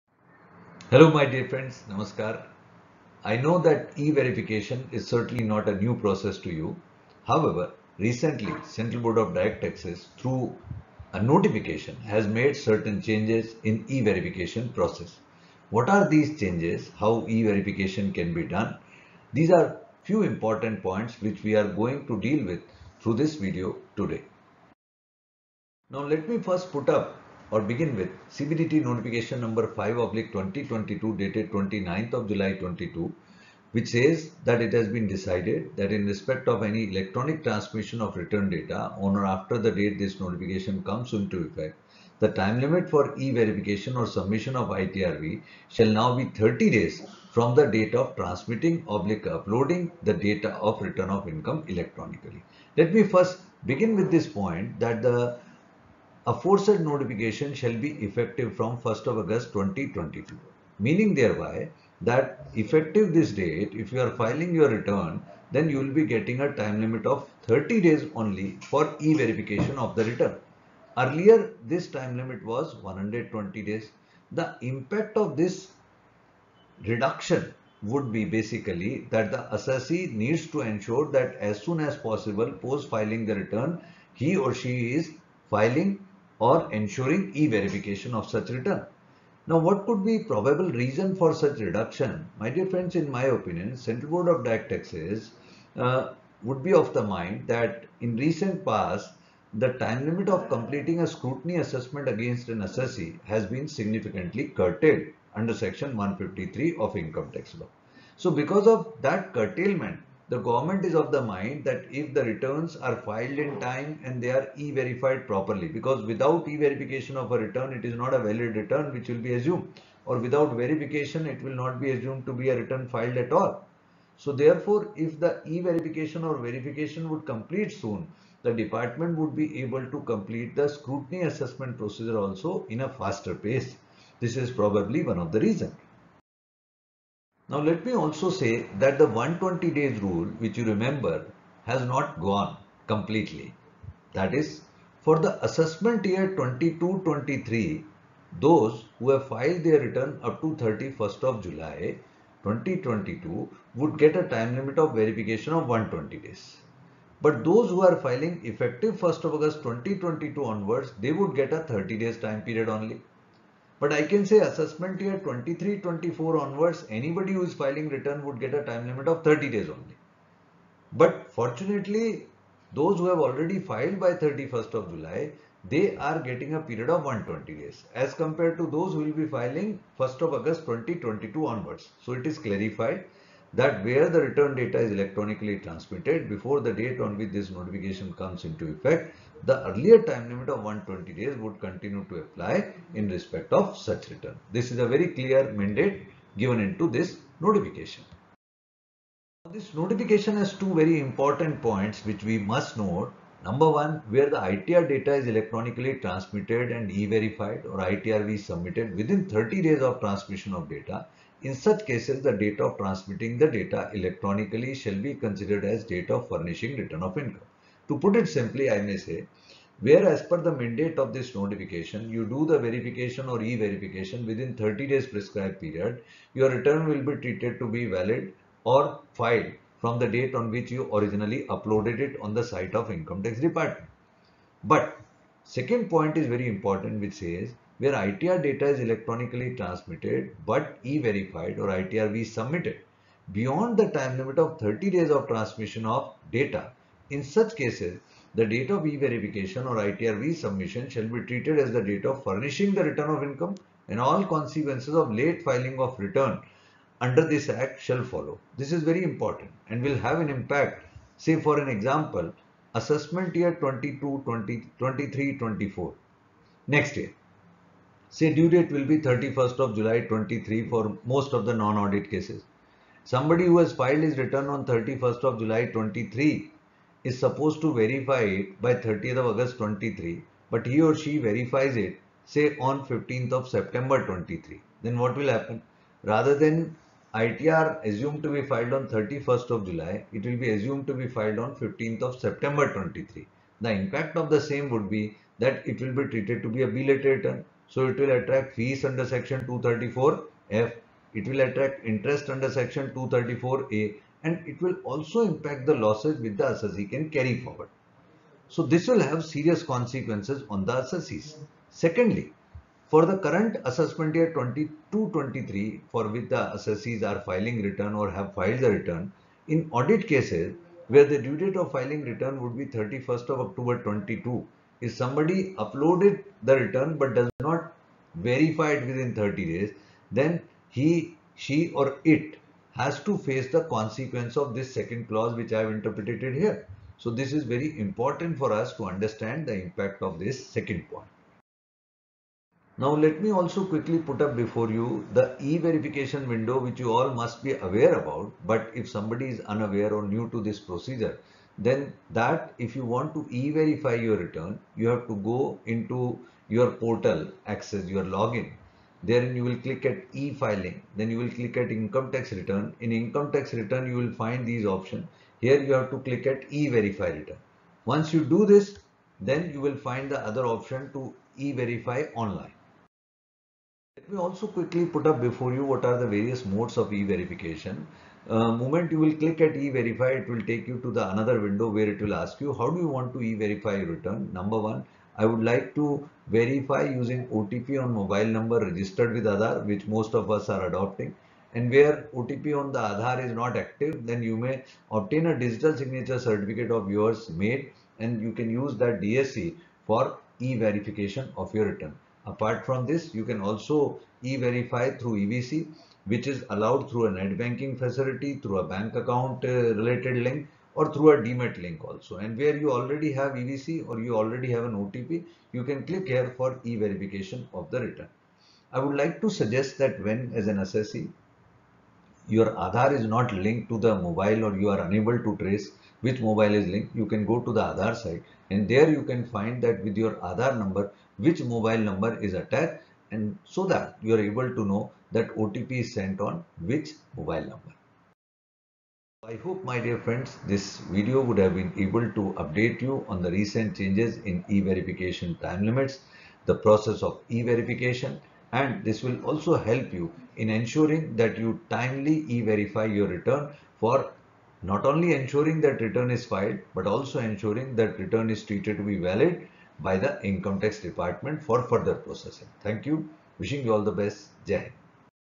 Have You E-verified your ITR (English Version) | CA Anoop Bhatia
Hello Friends, post filing of an ITR the e-verification / verification of such return is a very important step. The Central Board of Direct Taxes (CBDT) has recently vide0 notification no. 05/2022 has significantly curtailed the time limit of such verification. This video accordingly is a discussion on such curtailment and it’s impact of assessees at large. In addition to it the video also has a discussion on how to e-verify your ITR. I hope you will find this video useful and effective. Your precious suggestions and questions are
most welcome.

e-verification
CbdtNotificationNo05/2022